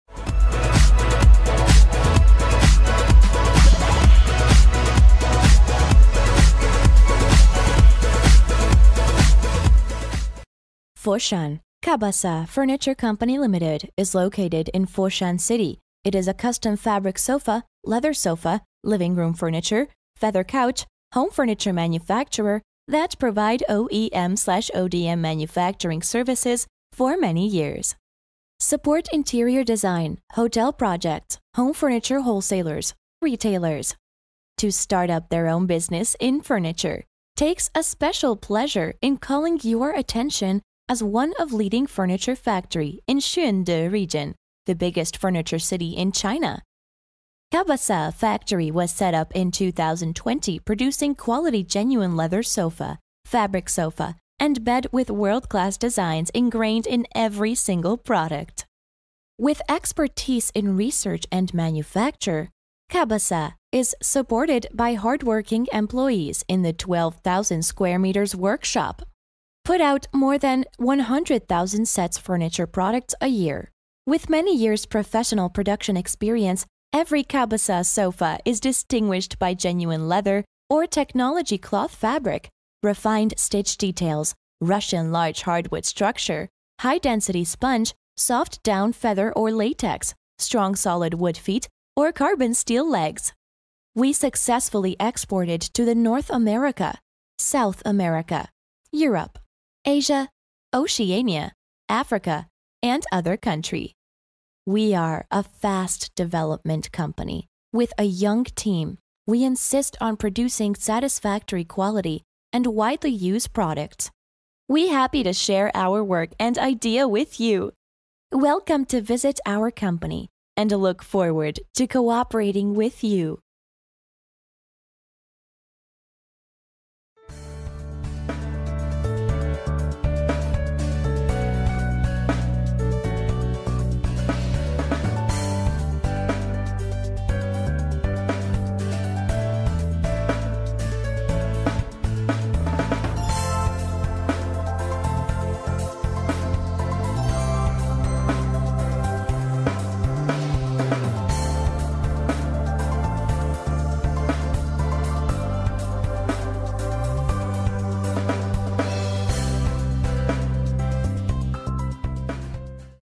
Kabasa furniture factory, one of the leading China sofa manufacturers
Kabasa is a custom sofa manufacturer that provide OEM/ODM manufacturing services for 15 years. Suport Interior design, Hotel Projects, Home furniture Wholesalers, Retailers.to start up their own business in furniture.